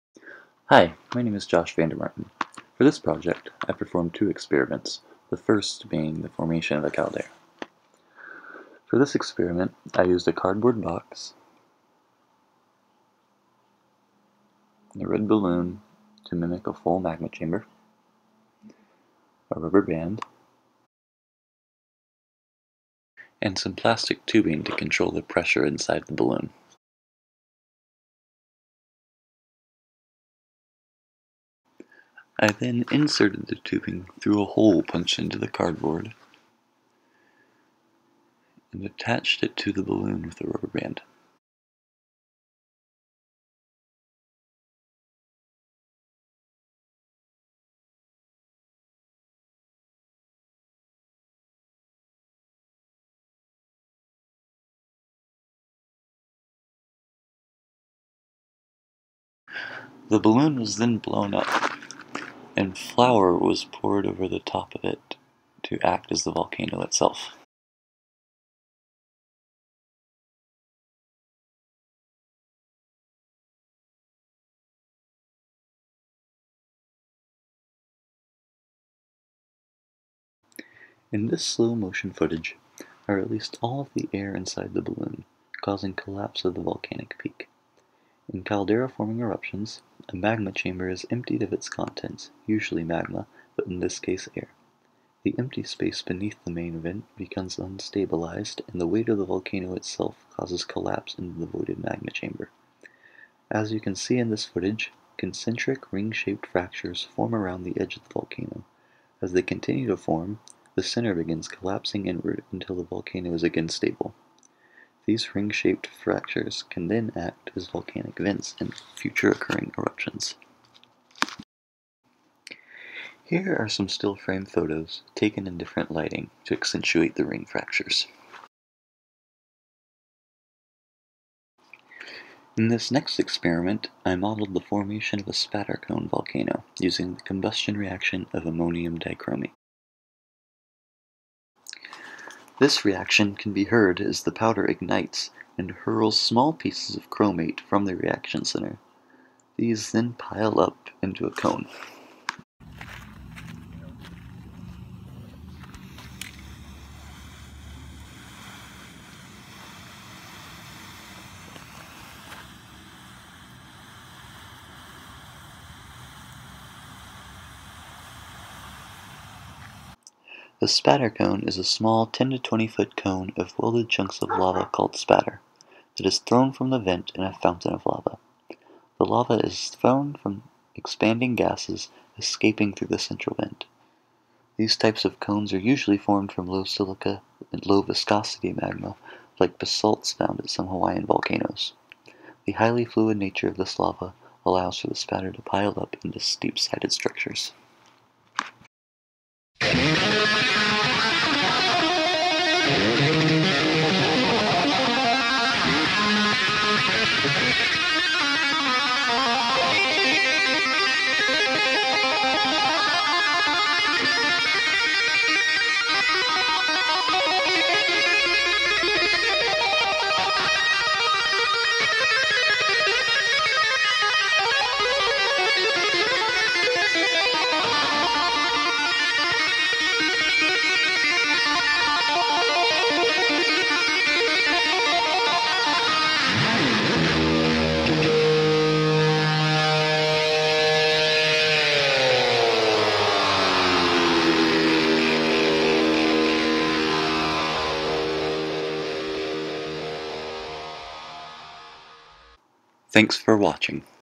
Volcanology Project
Models for the formation of caldera and spatter cone structures.